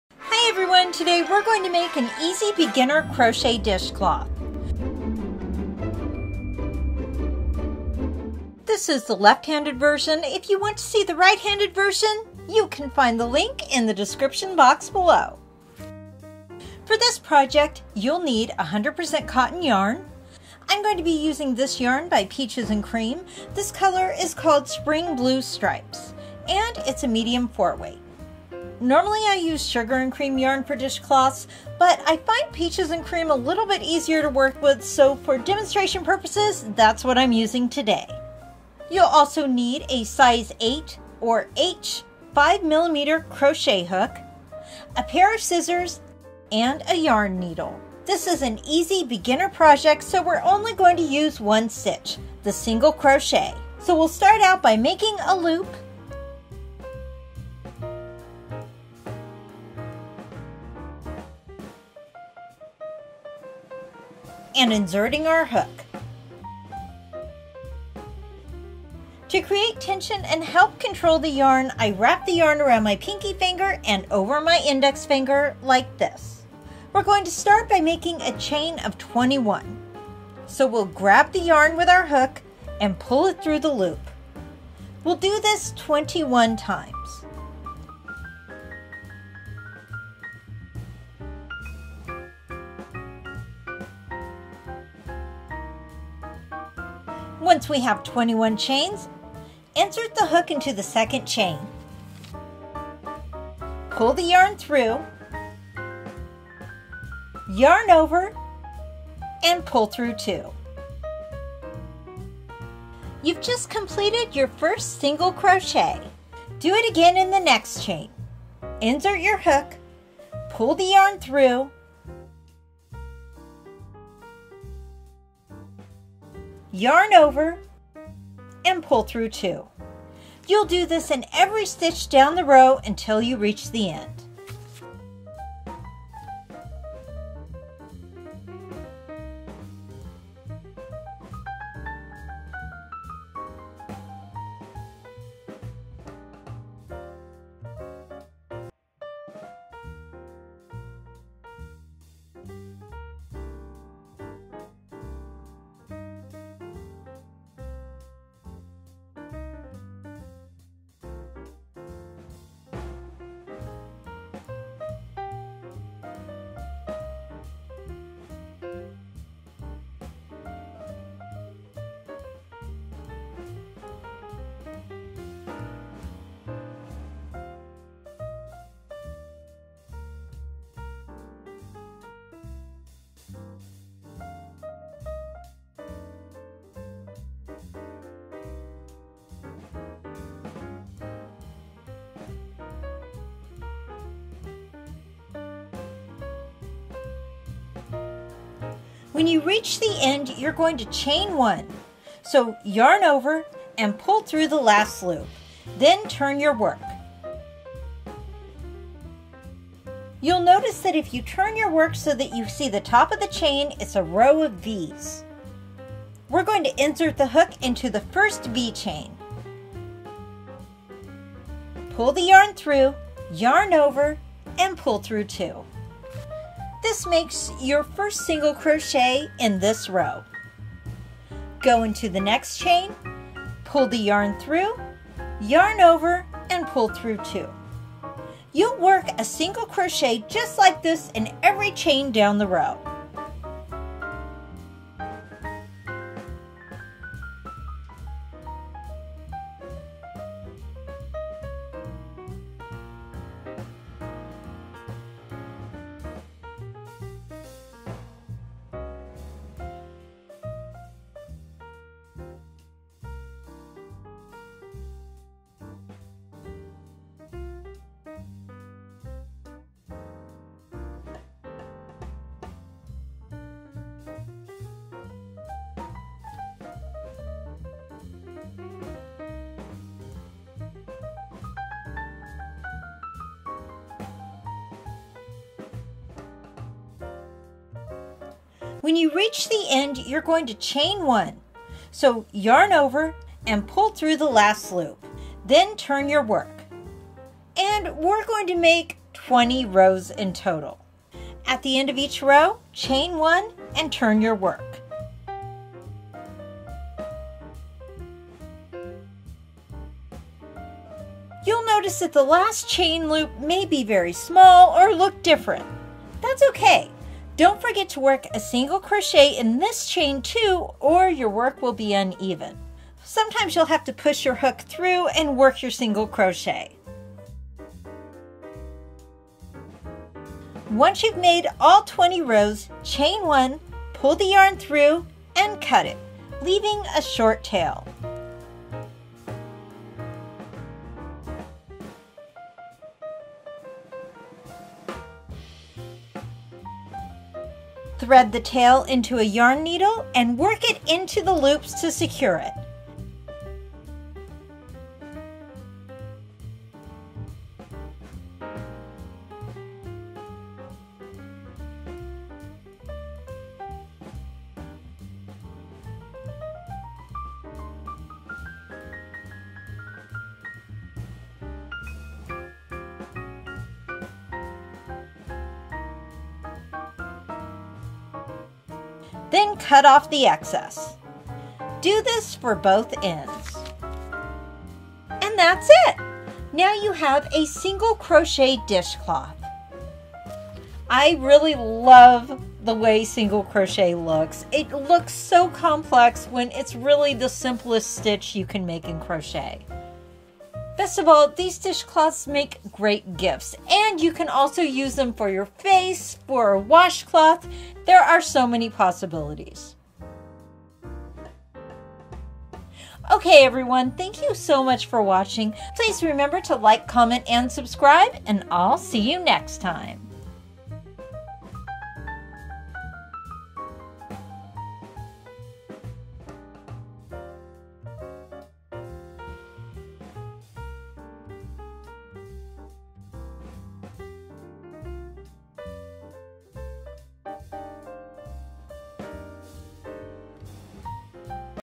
Easy Beginner Single Crochet Dishcloth - How To Make a Crochet Dish cloth - Left Handed Version
How to make a crochet dishcloth left handed version. This is an easy beginner dishcloth that anyone can make. It uses only one stitch: the single crochet. This is an easy beginner crochet project